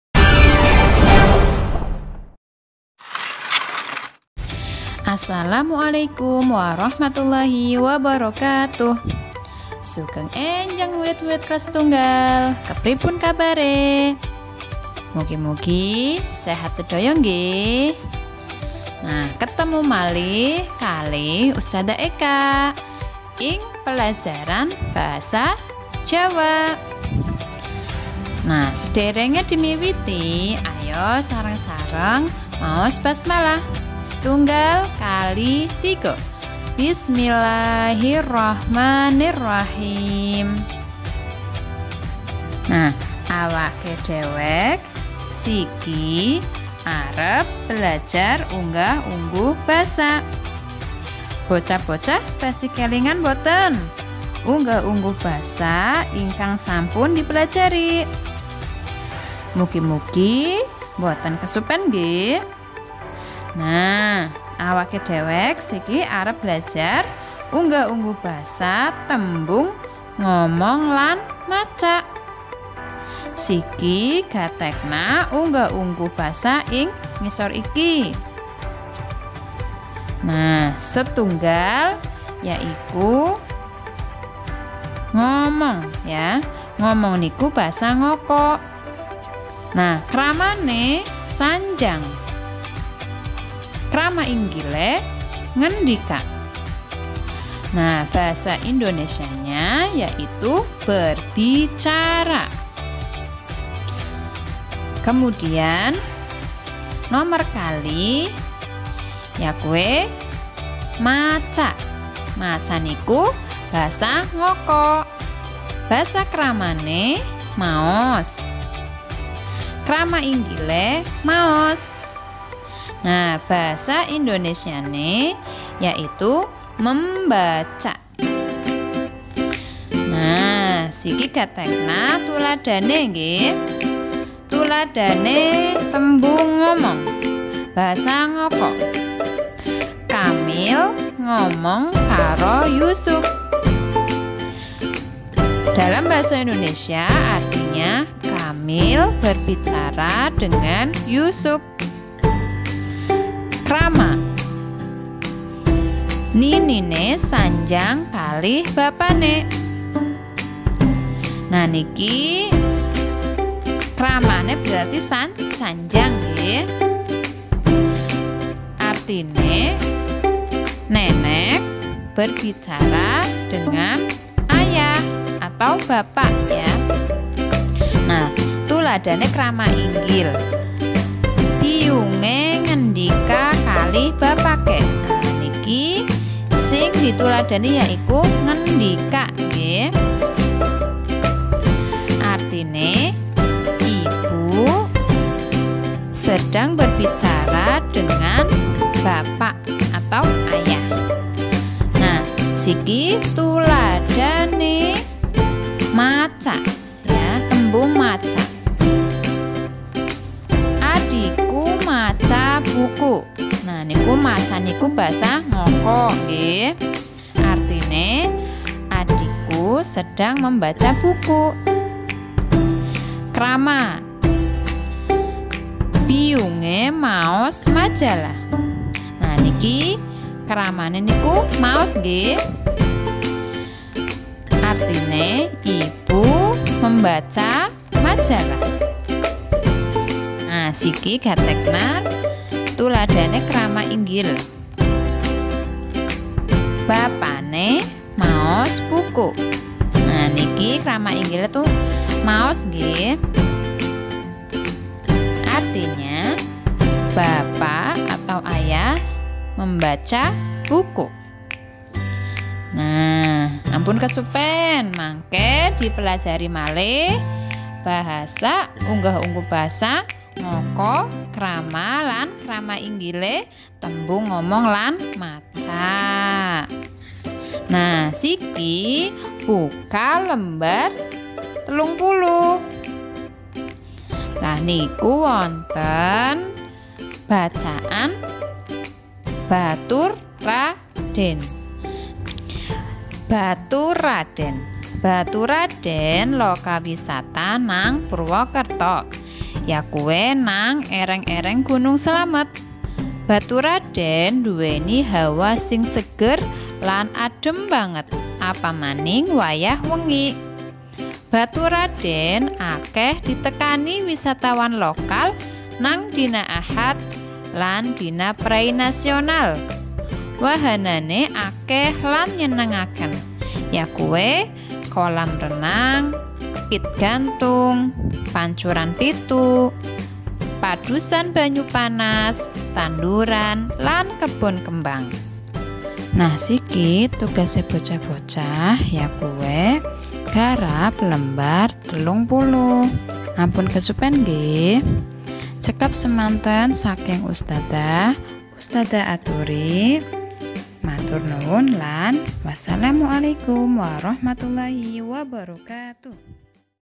Kelas 1 : Bahasa Jawa Tema 3 Pekan 2 | SD Al Irsyad Al Islamiyyah 02 Purwokerto
pelajaran bahasa jawa kelas 1 tema 3